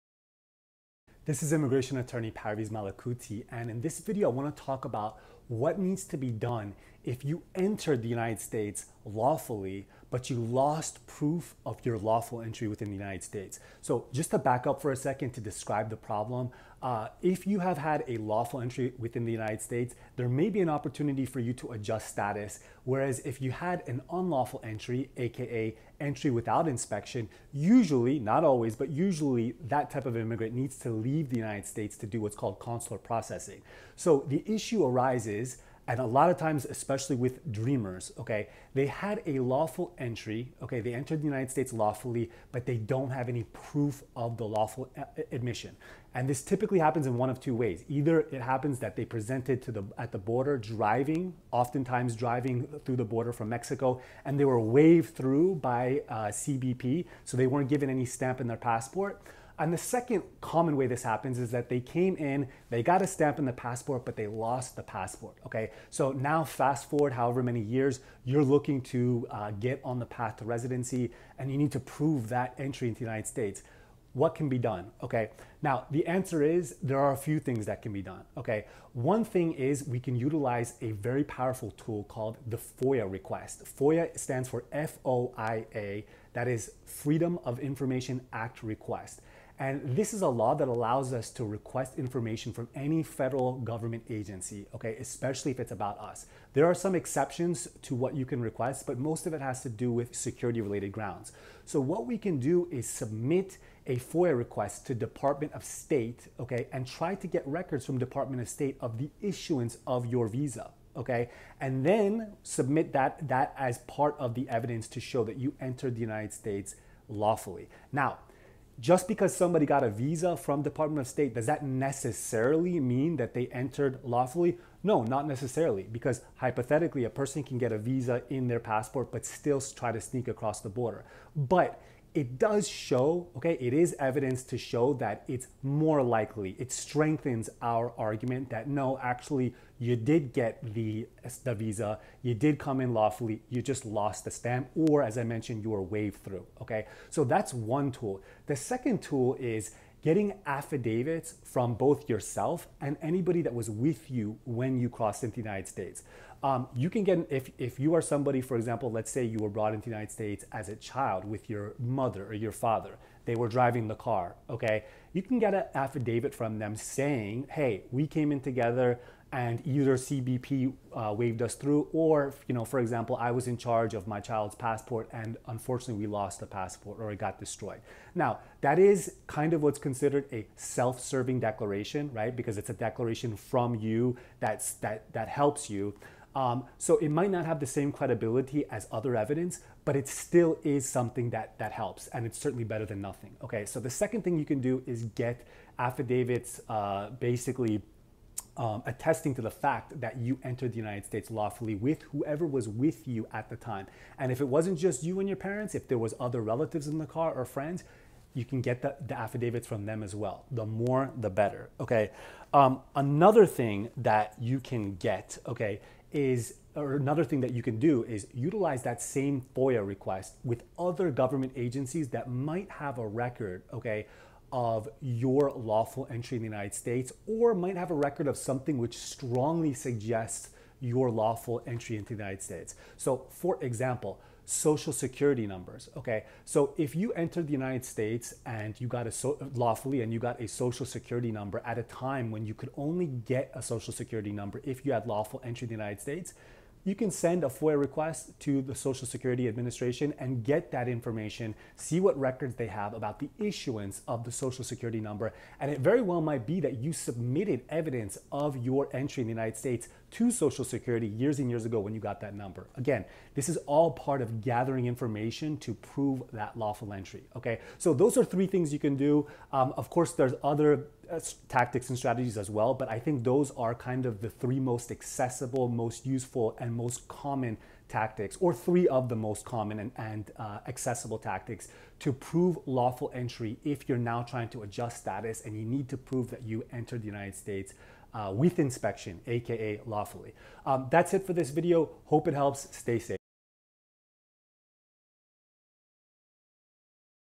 How to Prove LAWFUL ENTRY for Immigration (AOS)
Usually in order to adjust status, one has to prove lawful entry into the United States aka entry "with inspection."  In this video, Immigration Attorney Parviz Malakouti talks about how to prove lawful entry when either:

1) you were "waived in" across the border by CBP without a stamp in your passport or an I-94; or
2) Your passport was stamped and/or you were given an I-94 but you LOST the documents.

Attorney Marketing

Law Office of Parviz Malakouti
11620 Wilshire Boulevard, Suite 900